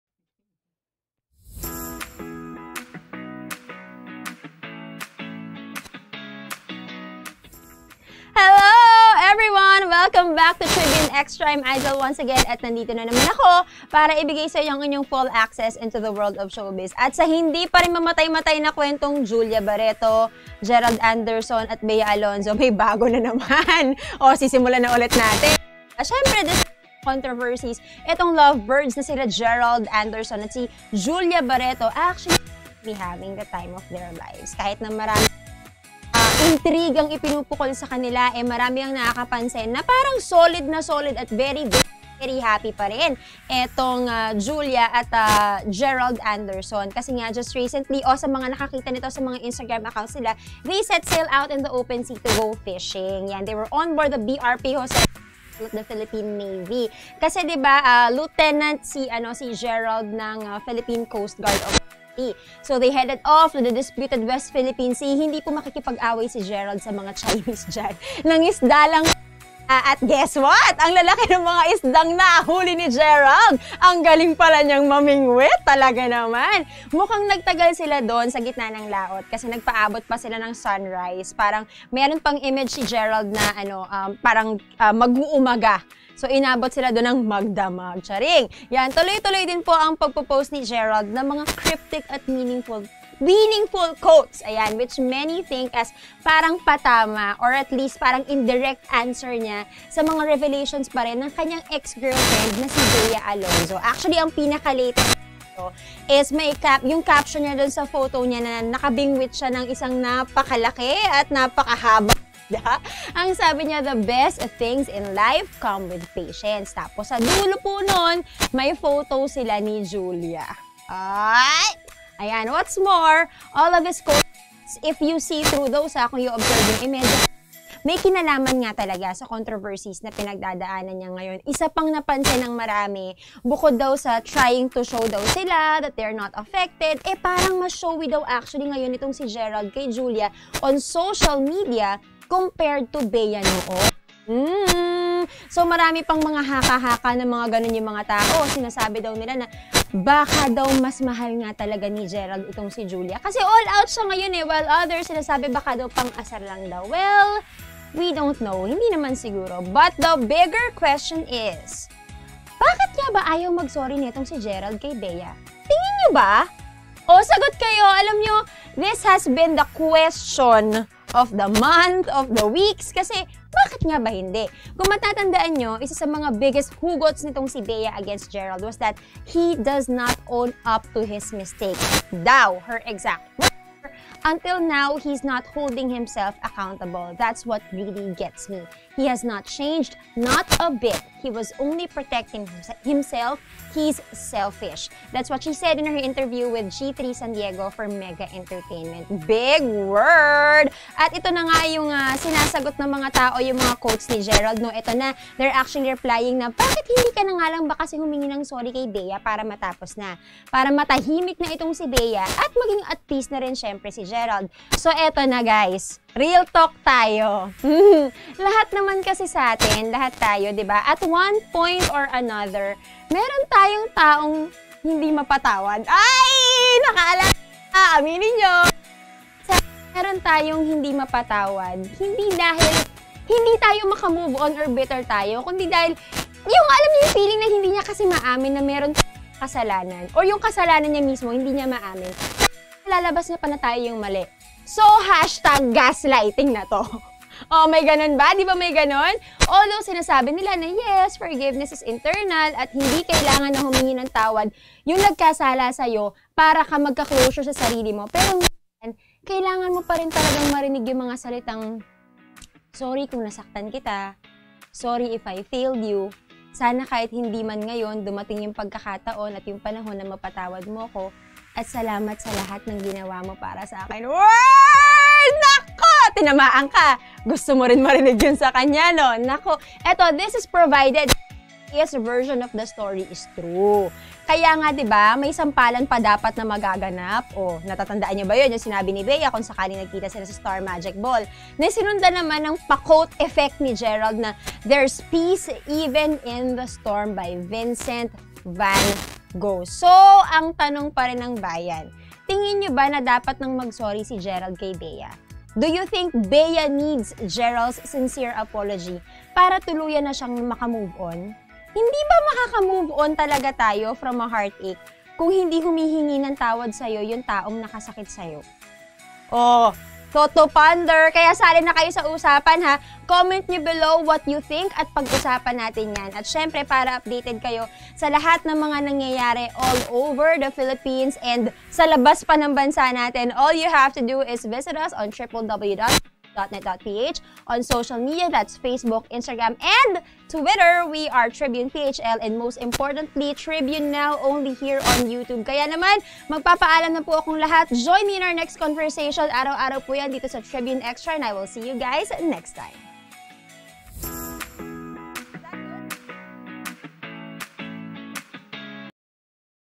Tribune Xtra 14 April 2021
Gerald Anderson and Julia Barretto go fishing. Will he ever ask for Bea Alonzo's forgiveness?